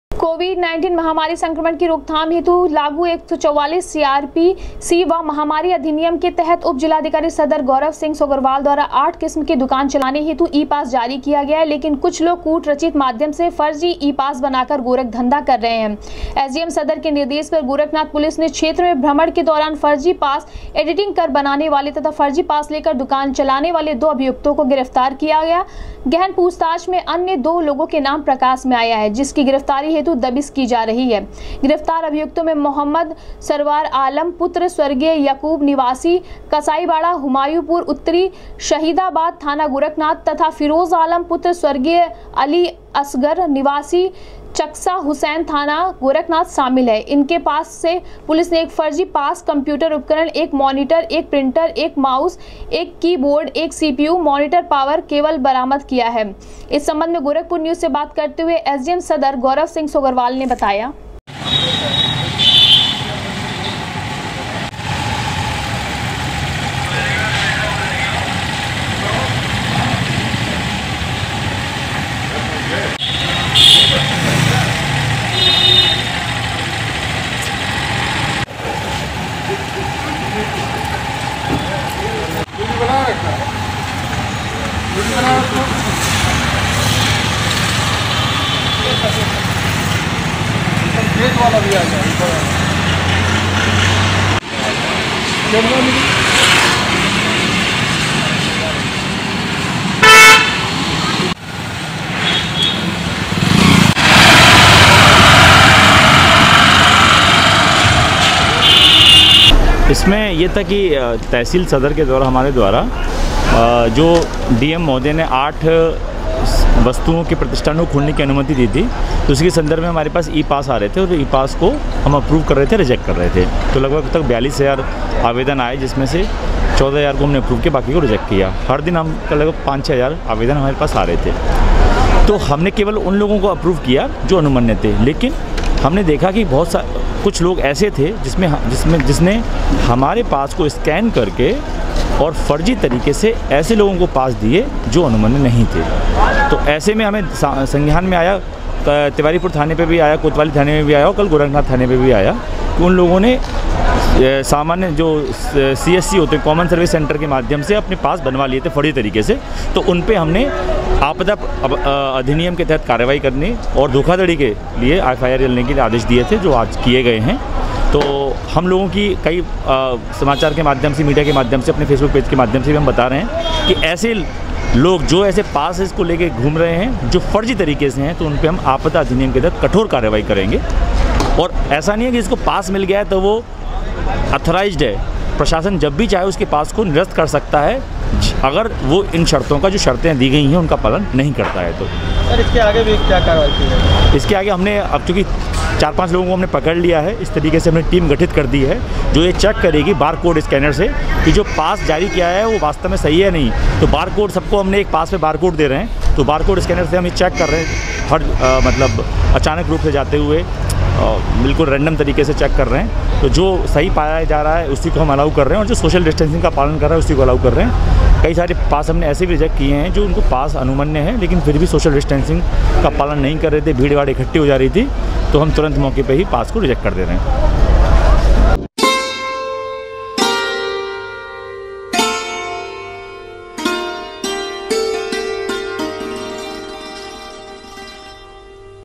एसडीएम सदर के निर्देश पर गोरखनाथ पुलिस ने कूटरचित फर्जी ई-पास बनाने वाले दो अभियुक्तों
एसडीएम सदर के निर्देश पर गोरखनाथ पुलिस ने कूटरचित फर्जी ई-पास बनाने वाले दो अभियुक्तों को किया गिरफ्तार, बरामद किये पास बनाने वाले उपकरण